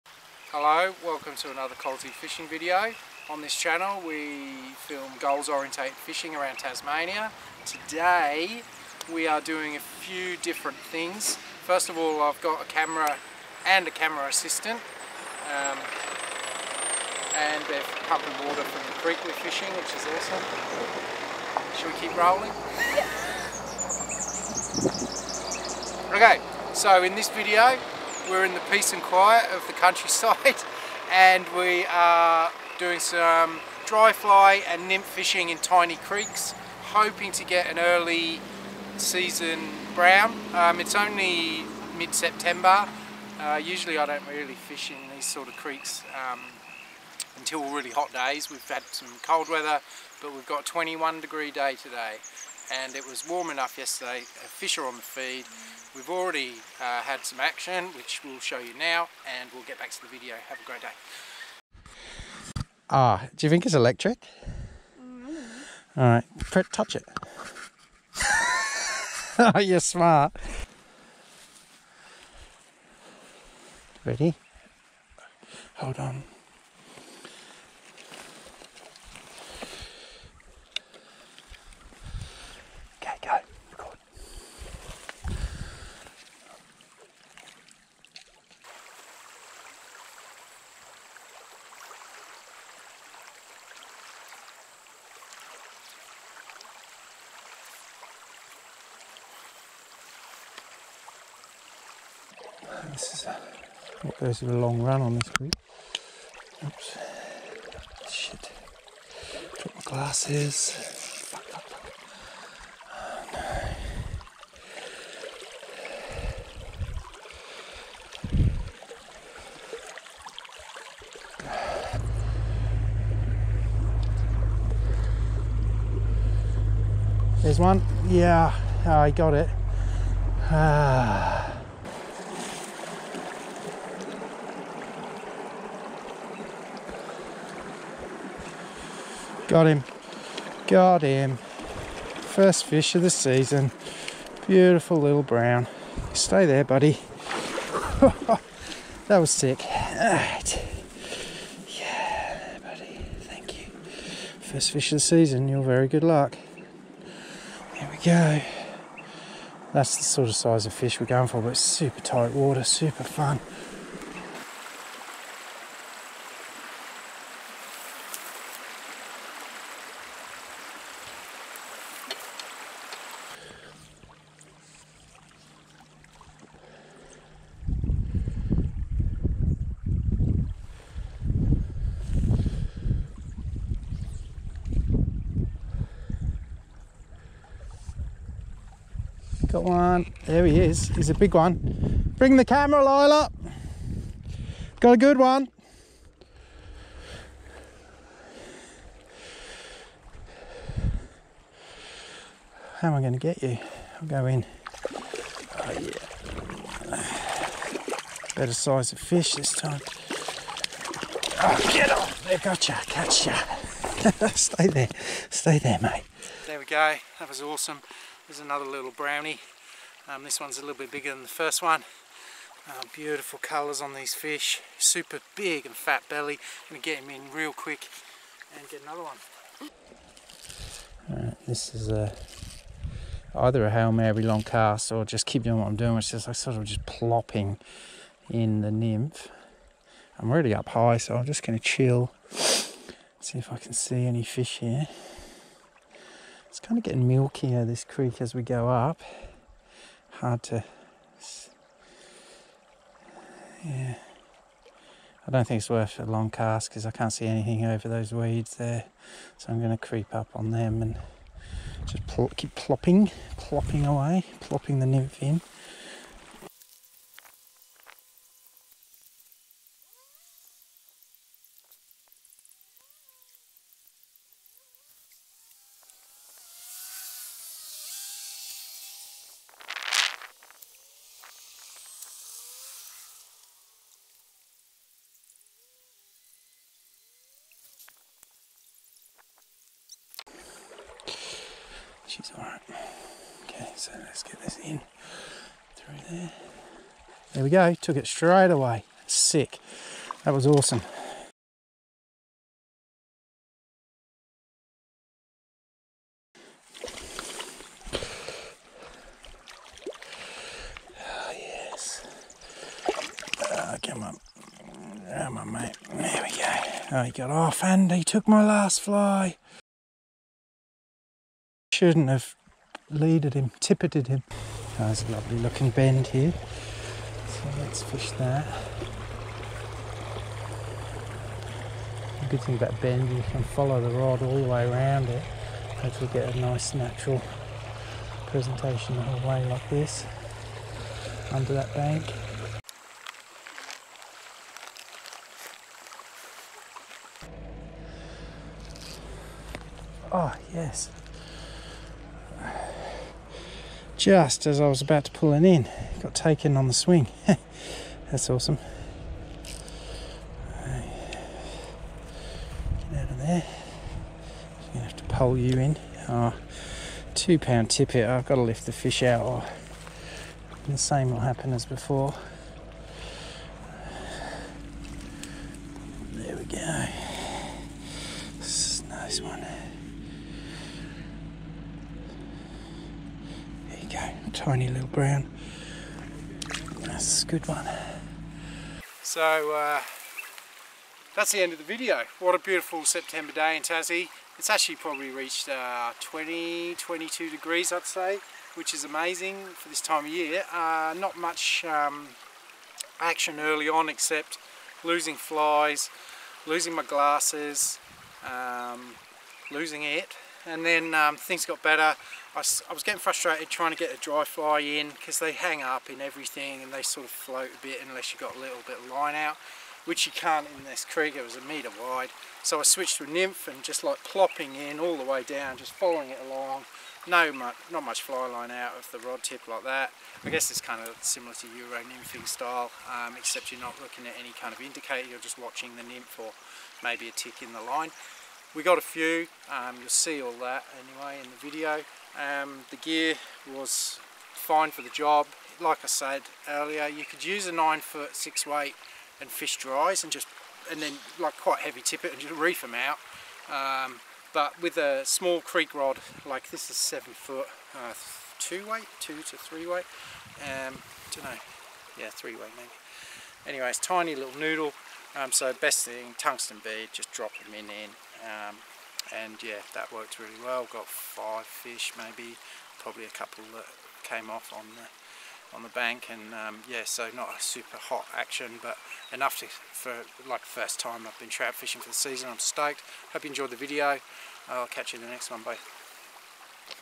Fly fishing tiny creek for brown trout, Tasmania
In this video I take a drive in NE Tasmania during an unseasonably warm day, I find a lovely creek, in some parts less than a metre wide, to target wild brown trout.  They didn't seem too fired up or want a dry fly but I get a few to come out of hiding to take a nymph.